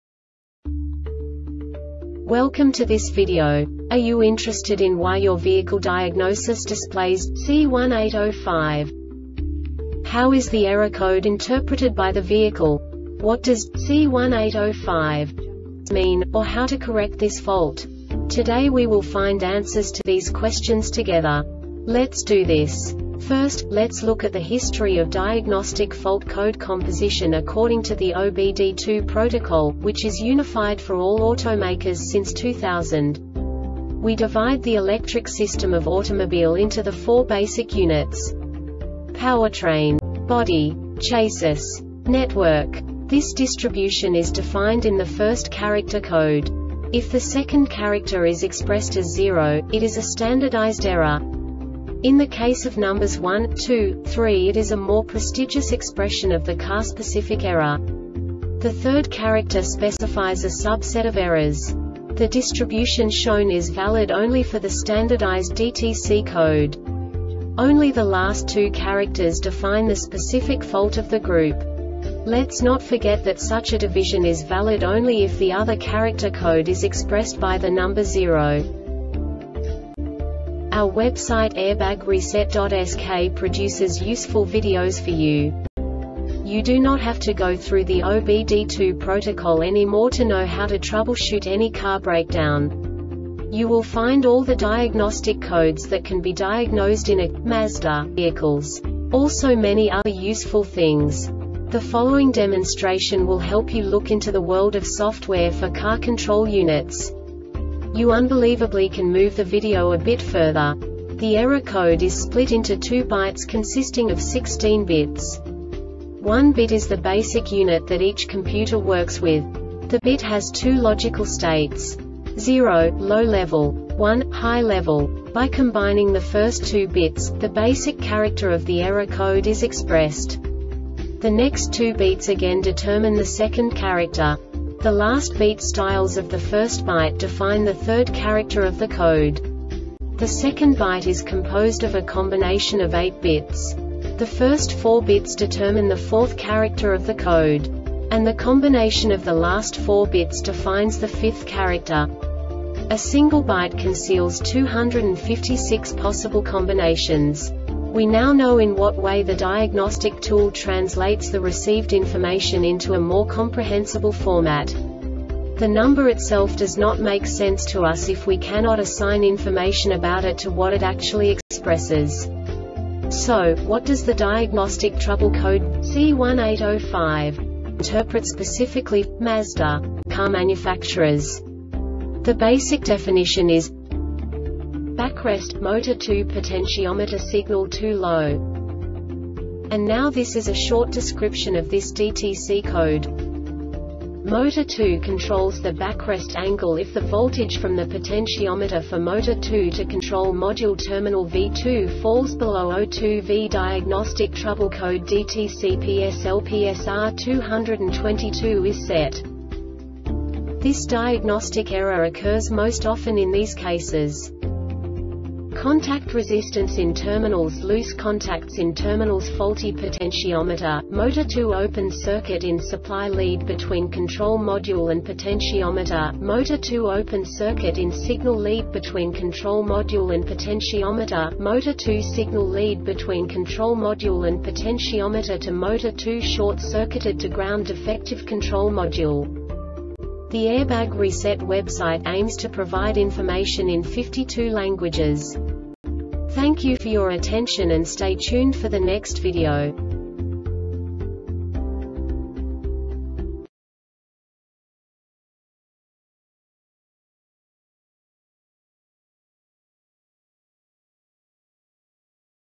DTC Mazda C1805 Short Explanation
Contents:
0:21 Basic DTC analysis according to OBD2 protocol standard.
1:48 Insight into programming
2:58 A diagnostically understandable description of the Diagnostic Trouble Code
3:05 Basic error definition

Make:
Mazda
Code:
C1805
Definition:
Backrest, Motor 2. Potentiometer Signal Too Low
Description:
Motor 2 controls the backrest angle.If the voltage from the potentiometer for motor 2 to control module terminal V2 falls below 0.2 V diagnostic trouble code (DTC) PSL or PSR-222 is set.
Cause:
Contact resistance in terminals.Loose contacts in terminals.Faulty potentiometer,motor 2.Open-circuit in supply lead between control module and potentiometer,motor 2.Open-circuit in signal lead between control module and potentiometer,motor 2.Signal lead between control module and potentiometer to motor 2 short-circuited to ground.Defective control module.